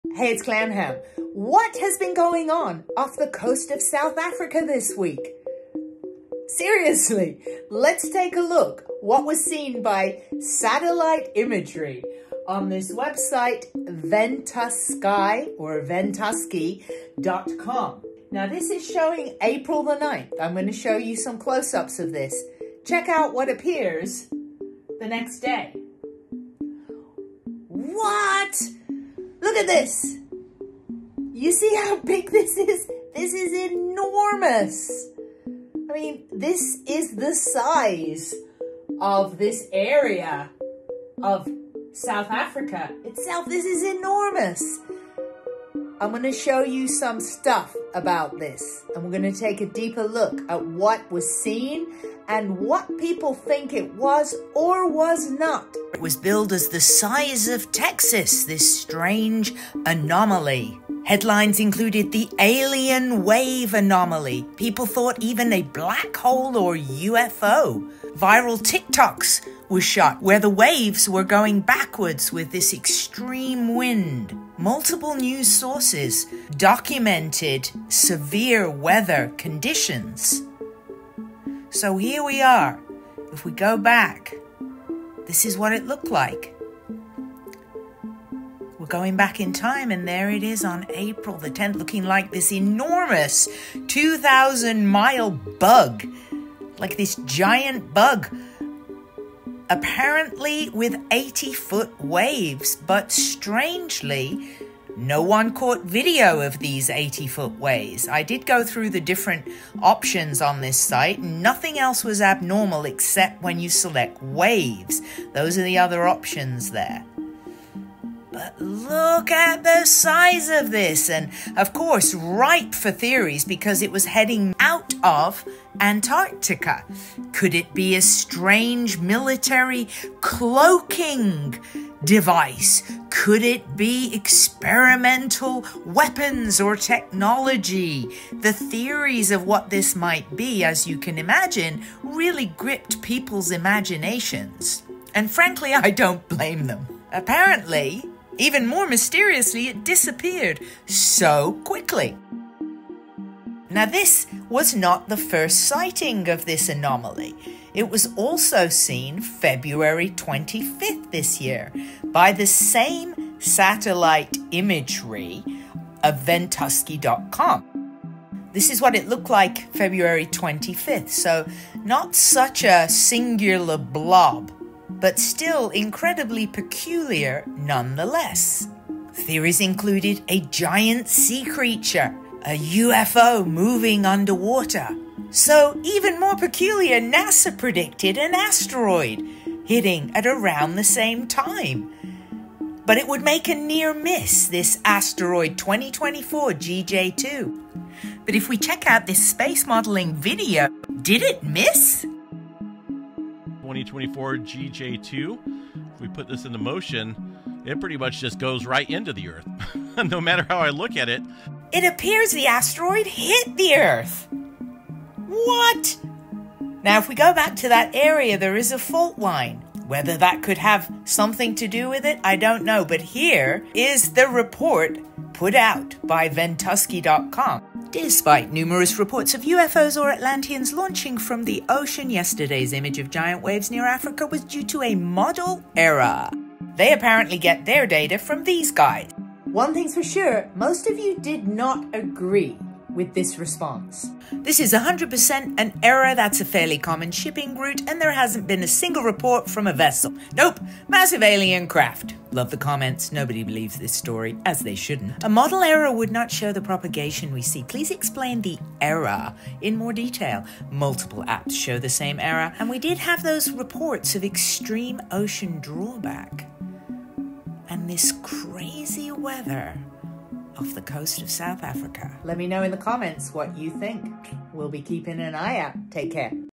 WHAT THE HECK WAS THIS???
What was the strange satellite image anomaly seen on the coast of South Africa this week?